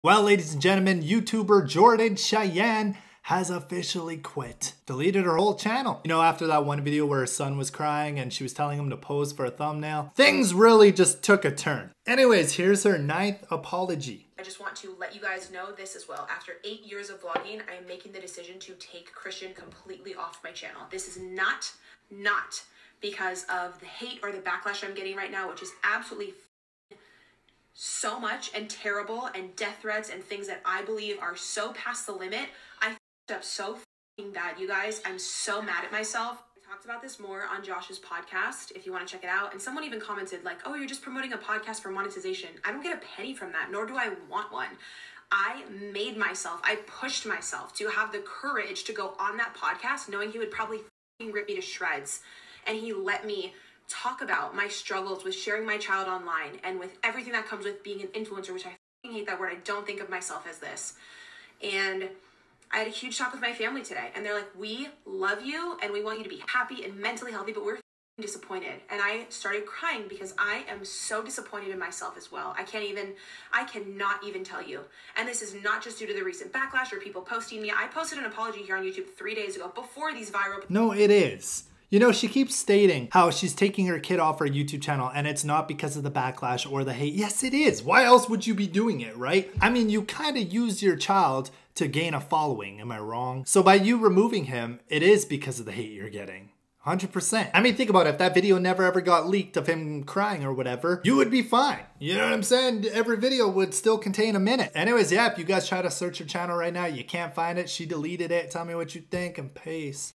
Jordan Cheyenne QUITS YouTube!! (LEAKED VIDEO)
Jordan Cheyenne officially QUITS Youtube after this LEAKED apology from a private story..